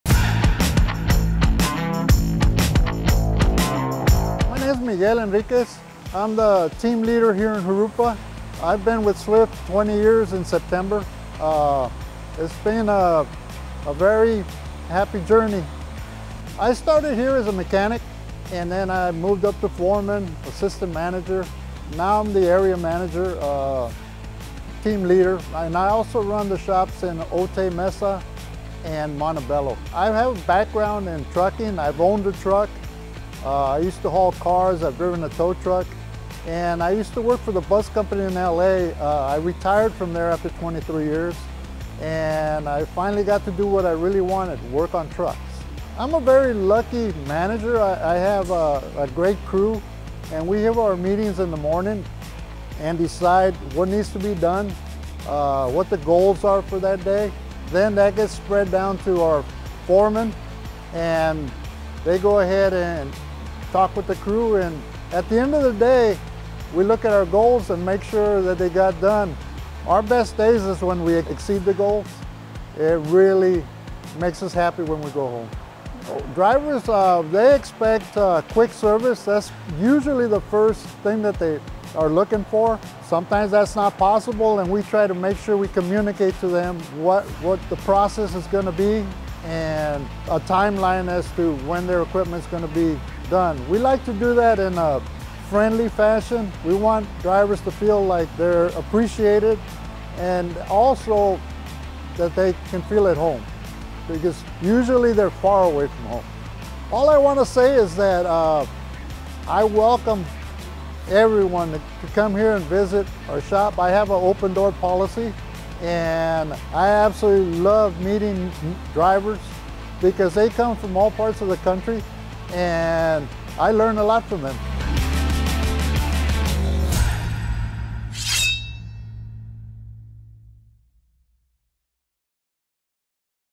Meet Miguel | Shop Team Leader in Jurupa Valley
Join us for an on-site interview with Miguel Enriquez, the shop leader at Swift Transportation in Jurupa Valley. In this insightful conversation, we will delve deep into Miguel's experiences and expertise, gaining valuable insights into the world of transportation and shop operations.

Miguel Enriquez has been with Swift Transportation for nearly 20 years and has worked his way up from a technician to a leadership role. He is responsible for managing the shop operations and ensuring the timely maintenance and repairs of the company's fleet of trucks and trailers.